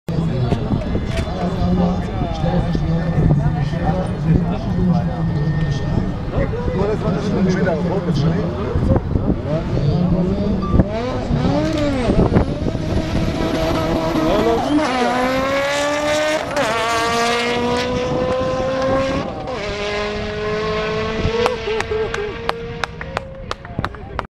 Israel 1st Official DRAG Race - Mitsubishi EVO Colt MV Racing VS JEEP Grand Cheerokee SRT8
EVO Colt time - 10.2 Sec
JEEP Grand Cheerokee SRT8 time - 14 Sex